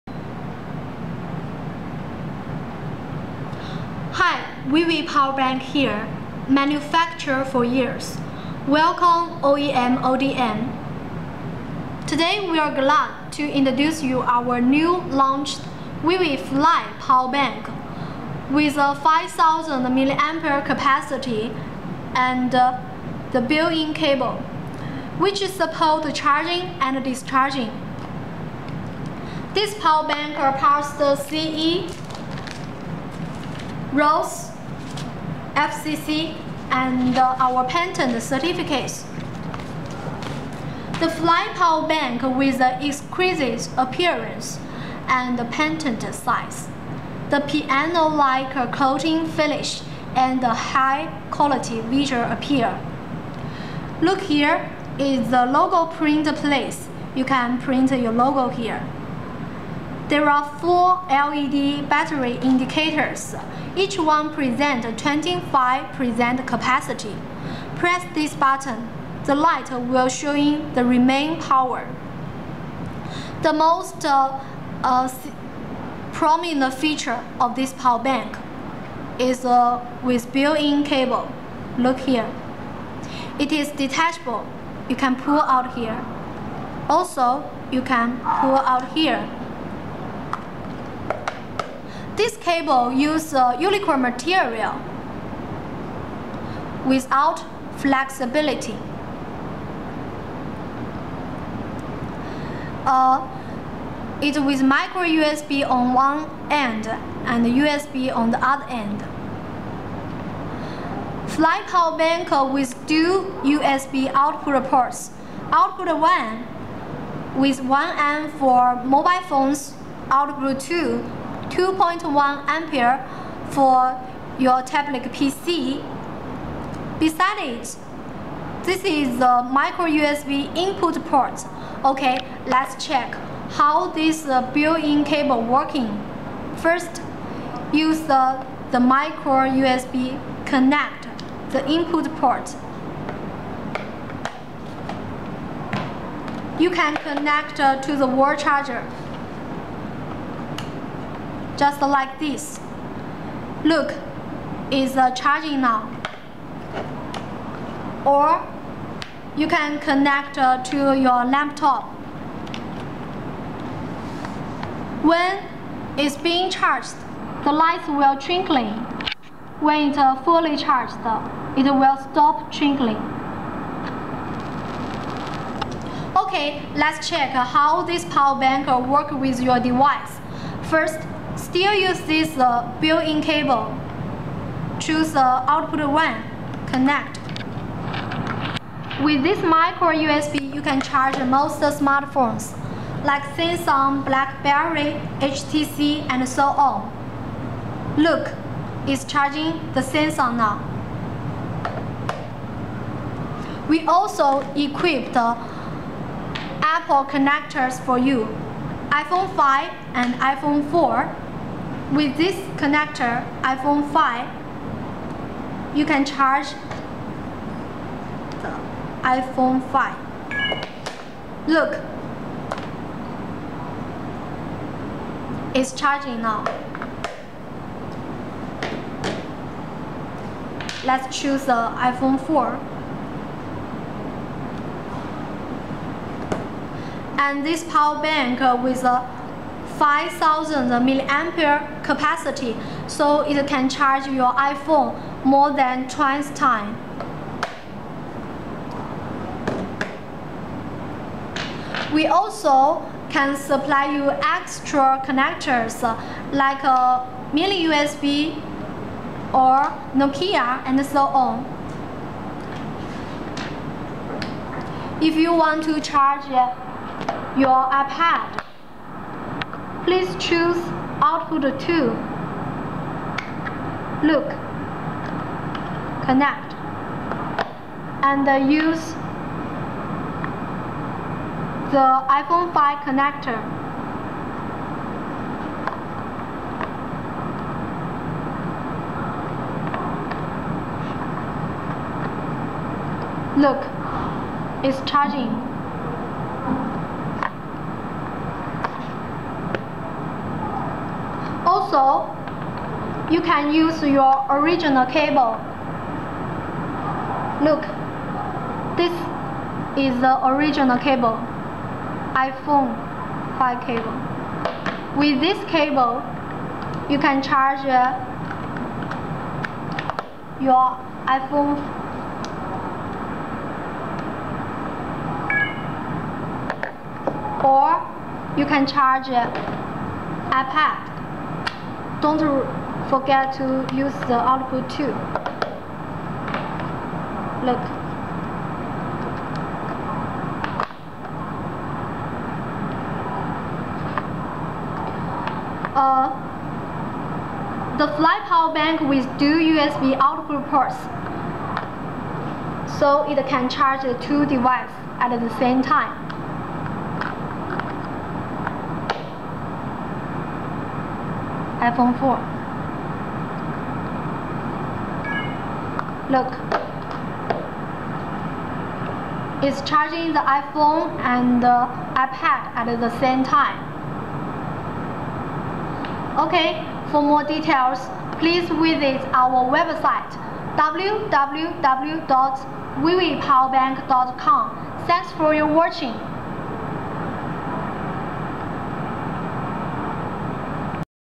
ViVi Fly 5000mah power bank charger
ViVi Fly power bank, 5000mah lithium polymer battery, dual usb output 5V 1A/ 5V 2.1A, detachable built-in usb cable which can support charging and discharging, portable charger for iphone4, iphone5, ipad, balckberry. samsung galaxy, ipad, tablet pc etc. Email to or call me @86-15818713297. Best regards